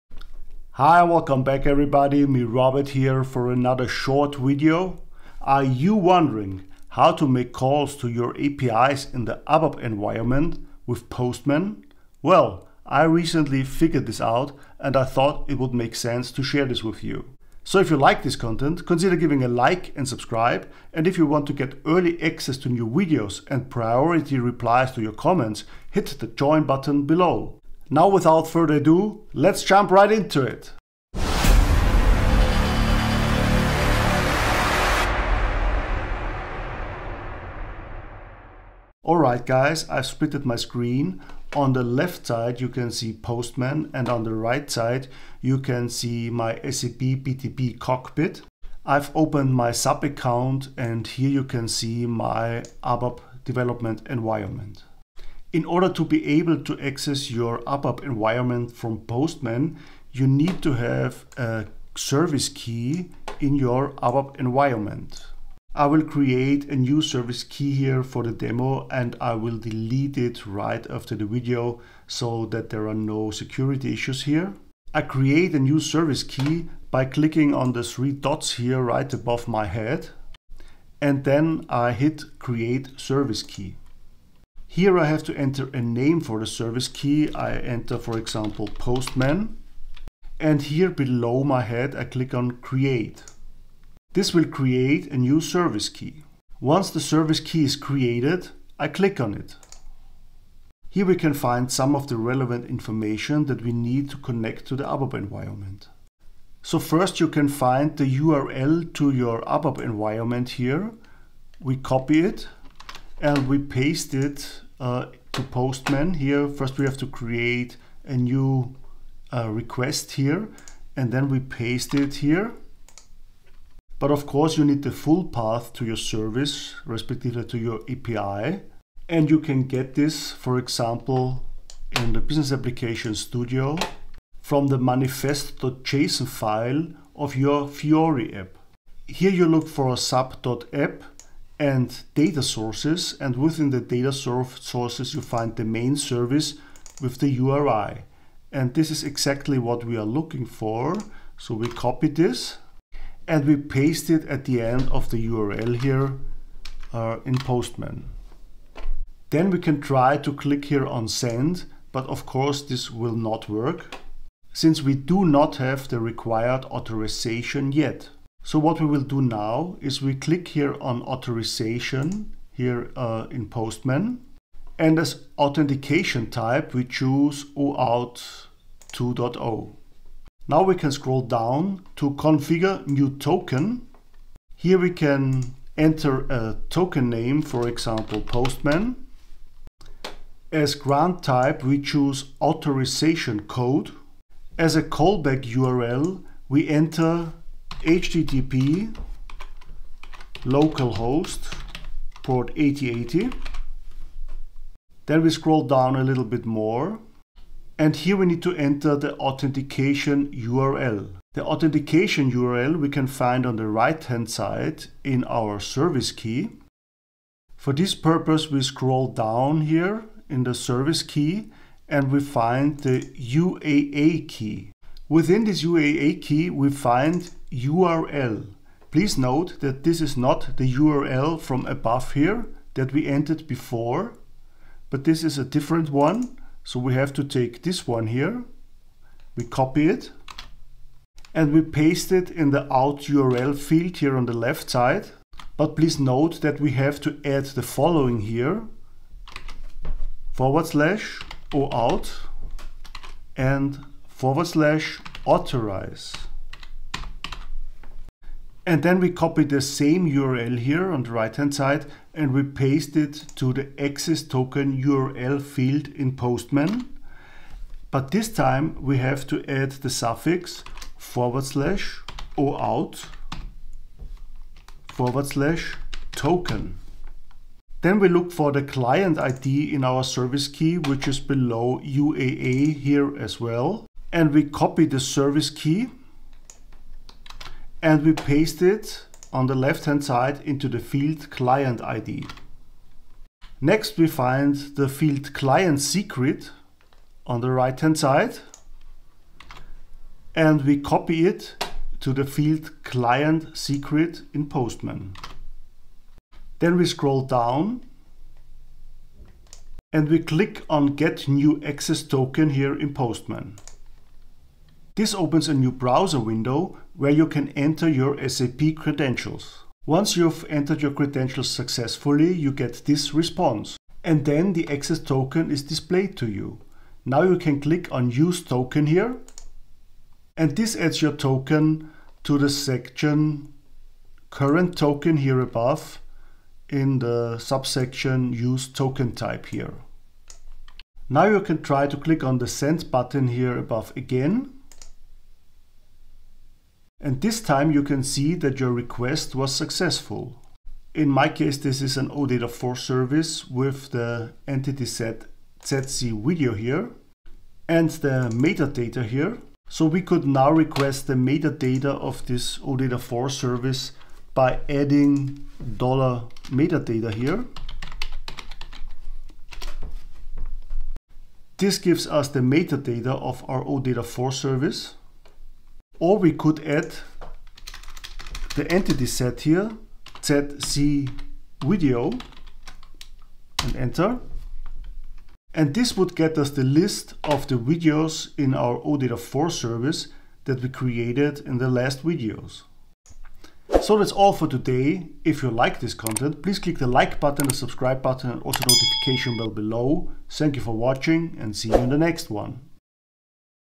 Calling APIs in ABAP Environment with Postman
In this video you learn how to call APIs in your ABAP Environment with Postman.

Chapters in this video:
0:00 - Intro
00:38 - Overview
01:14 - Create Service Key
01:46 - Postman Request
02:48 - OAuth 2.0 Authorization
05:06 - Get Access Token
05:40 - Request OData4 Service
06:33 - Outro